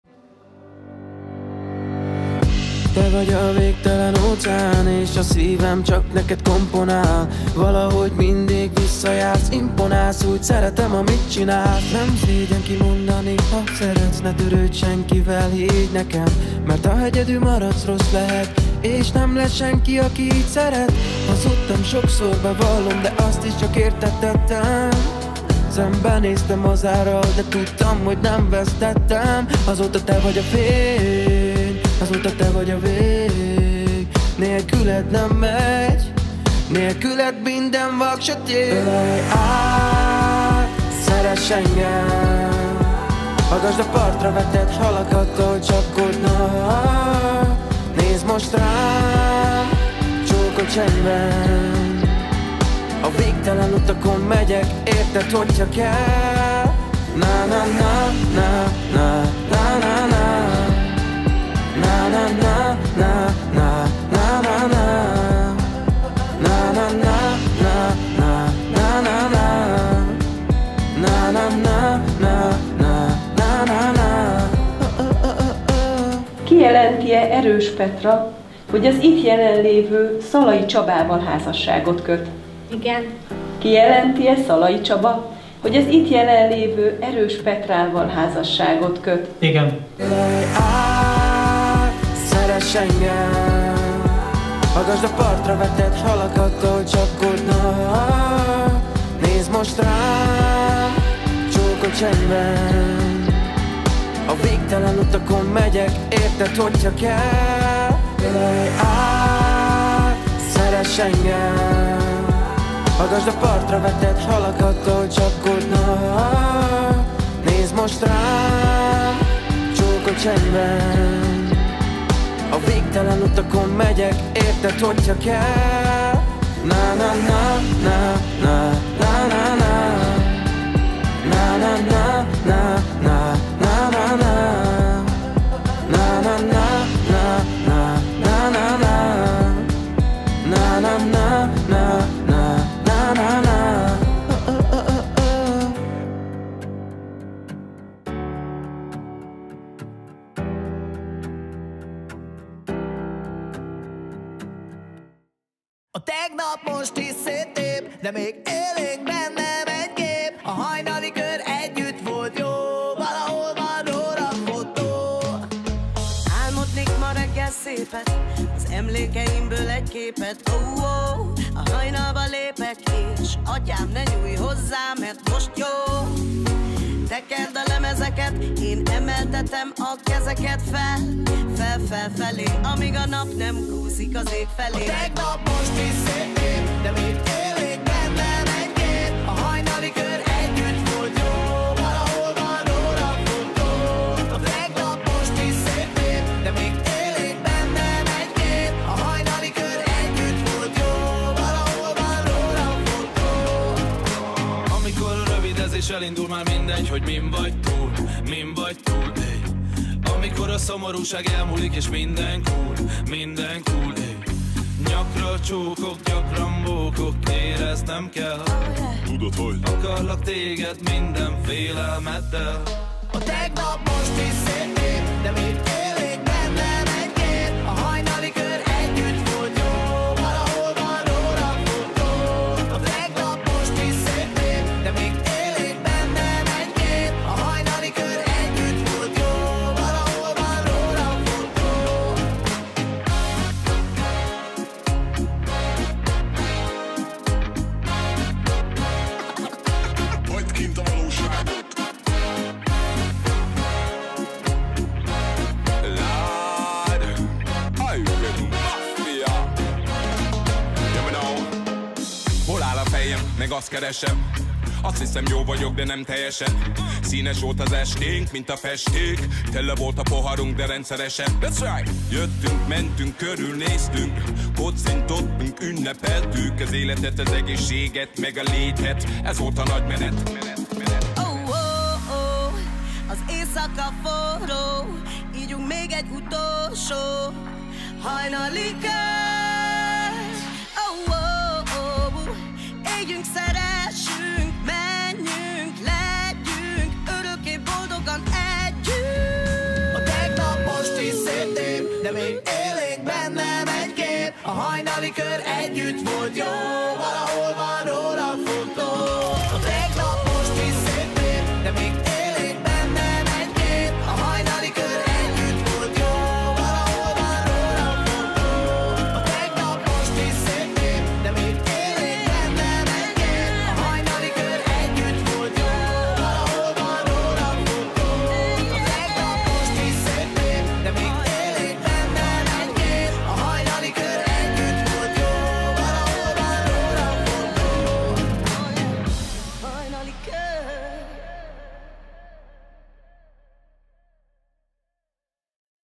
Petrus & Csabika esküvői videó | Wedding Film | 2022. szeptember 17.
Petrus & Csabika Wedding
2022. szeptember 17.
Helyszín: Pásztor Csárda Szombathely
Köszönjük mindenkinek, aki velünk ünnepelt!